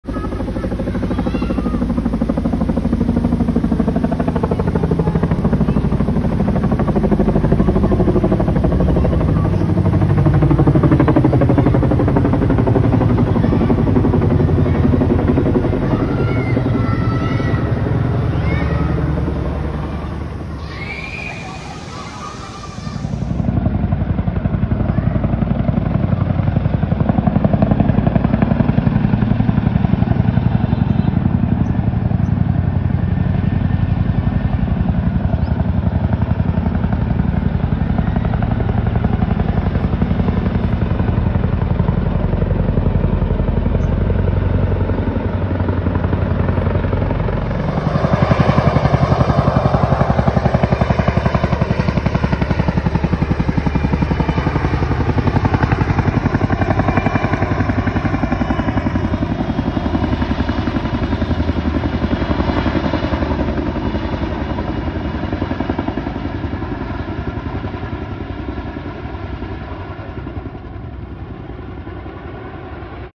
IAF CH-47 Chinook helicopter’s surprise landing at Adani Shantigram Cricket Ground, Ahmedabad
IAF CH-47 Chinook’s surprise landing at Adani Shantigram township’s Cricket Ground, Ahmedabad